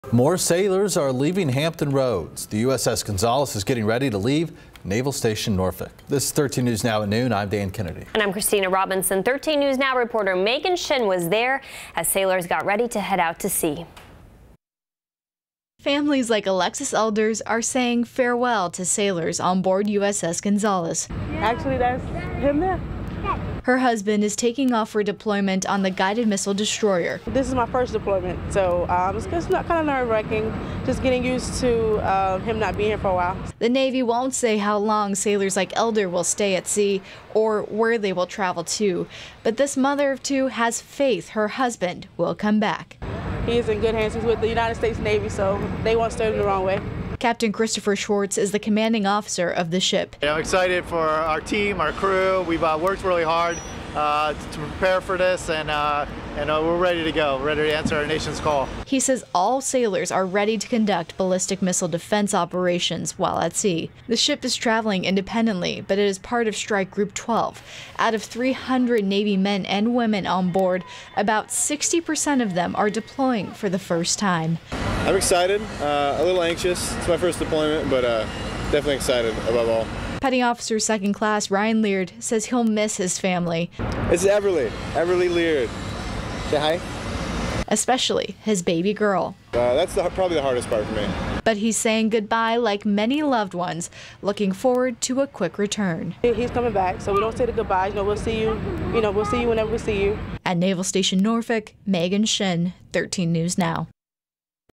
USS Gonzalez prepares for deployment
More sailors are getting ready to leave Hampton Roads.